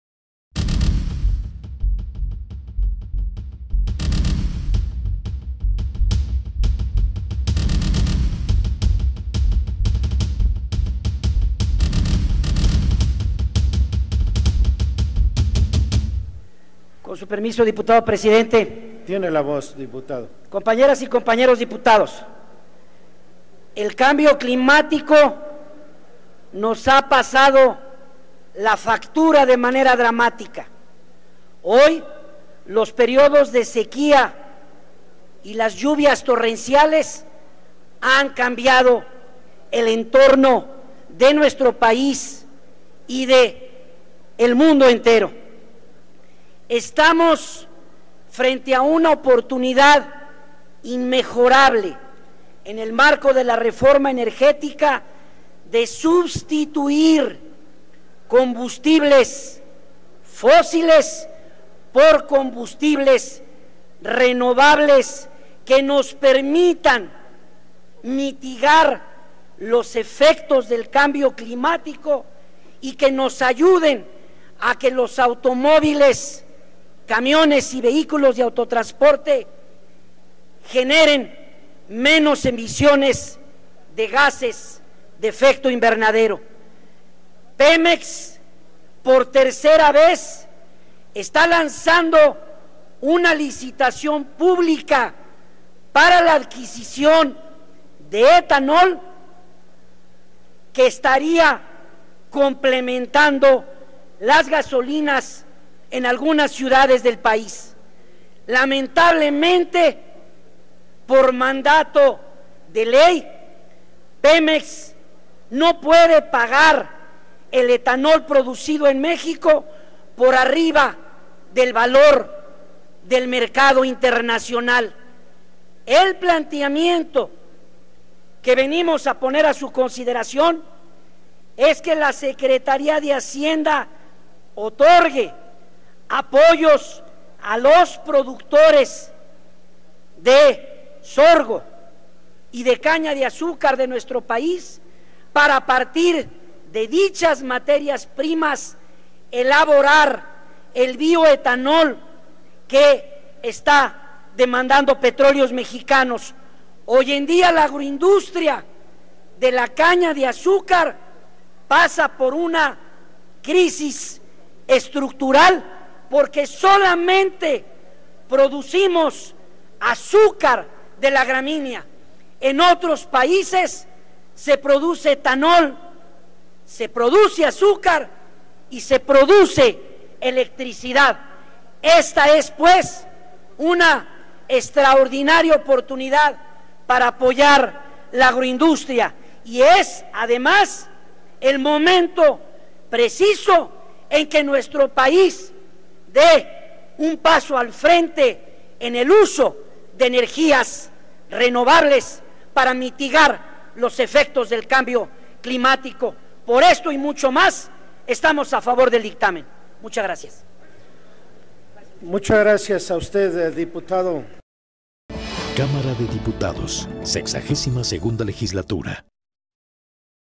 Dip. Salvador Romero (PRI) - Ley General de Cambio Climático
El Dip. Salvador Romero Valencia (PRI) considera que el cambio climático nos ha empezado a hacer daño, ya que tenemos temporadas de sequías o de lluvias torrenciales donde no las había, por lo que considera que estamos a tiempo de cambiar el uso de energías fósiles por el uso de energías renovables. Menciona que PEMEX está lanzando la licitación de adquisición pública de etanol que estaría sustituyendo a la gasolina como combustible, ya que PEMEX no tiene los recursos para comprar el etanol en el mercado internacional. Menciona que es una gran etapa para cambiar el uso de energías y es por ello que su grupo parlamentario votará a favor del dictamen.